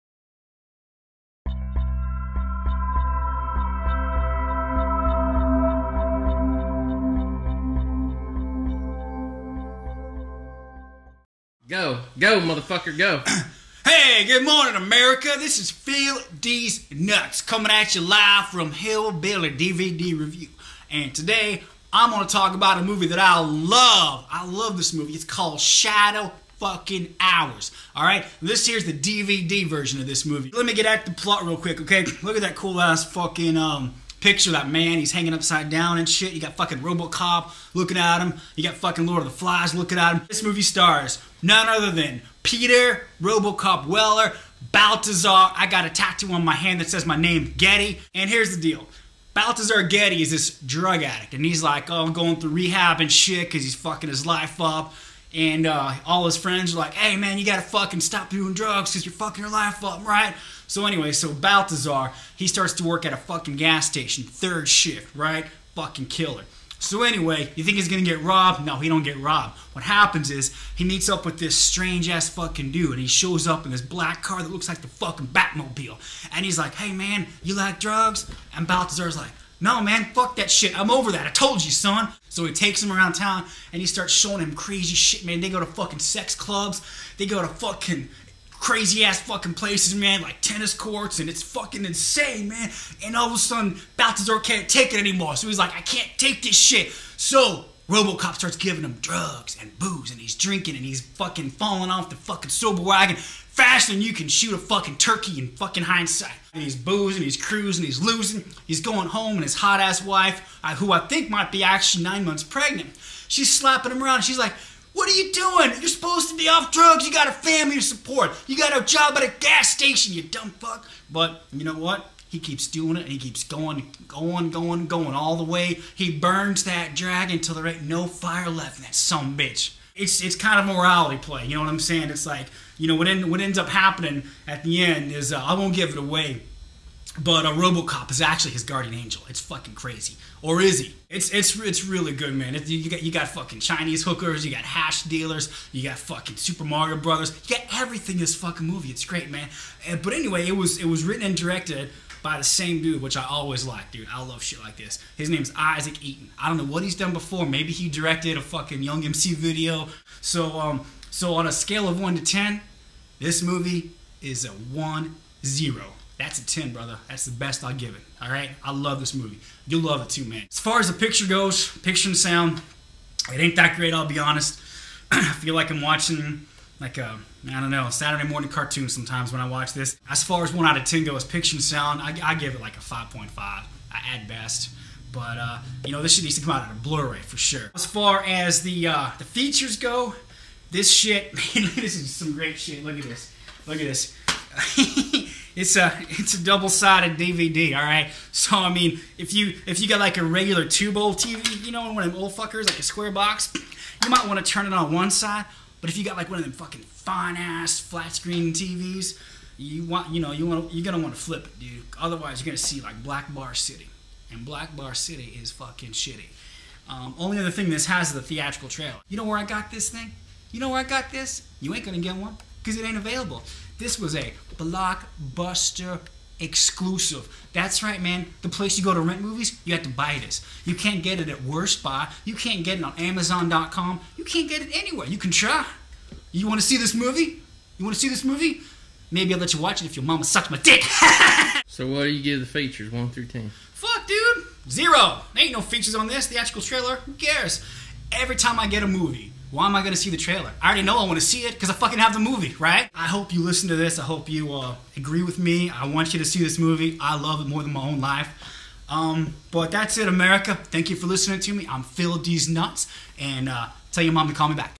SHADOW HOURS dvd review
Phil Dease reviews Shadow Hours (2000) starring Balthazar Getty, Peter Weller, Rebecca Gayheart, Brad Dourif,  Frederic Forrest, Downtown Julie Brown and directed by Isaac H. Eaton.

Have you seen this movie? Does the seedy underbelly of Los Angeles intrigue you? What did you think of it? Let us know by leaving a comment in the comments section or uploading a video response!